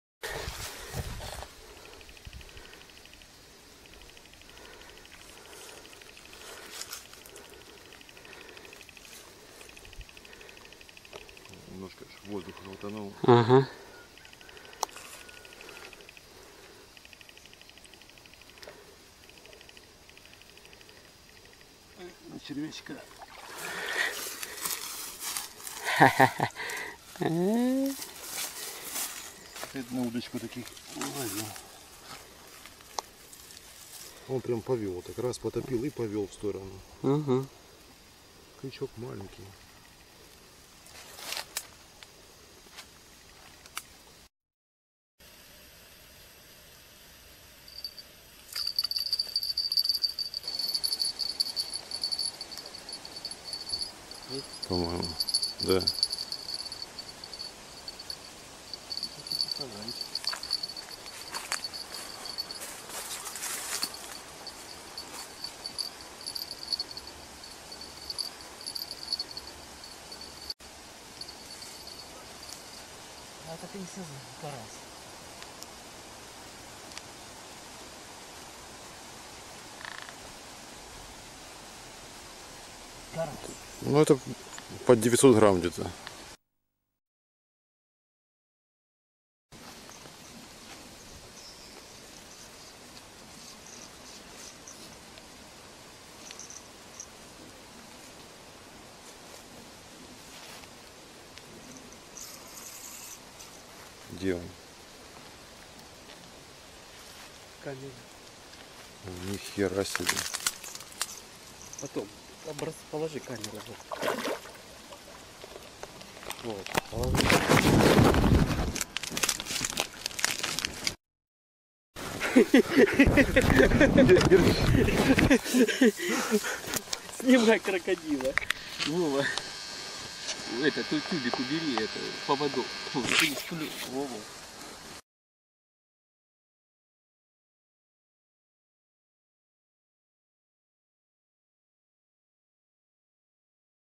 Улов Коллеги !!! Ловили карася , но бывают и случайности :) Карп на донку, наживка:червь+кукуруза.
Улов Коллеги !!! а Ловили карася , но бывают и случайности :) Карп пойман на снасть с кормушкой, наживка: червь+кукуруза. Для профи может это привычное дело таких ловить, а для простого рыбака это удача, отсюда и такие естественные, радостные эмоции :) .….((Мой канал- это (в основном) канал ЛЮБИТЕЛЯ-рыболова или рыбалка- как вижу ее я !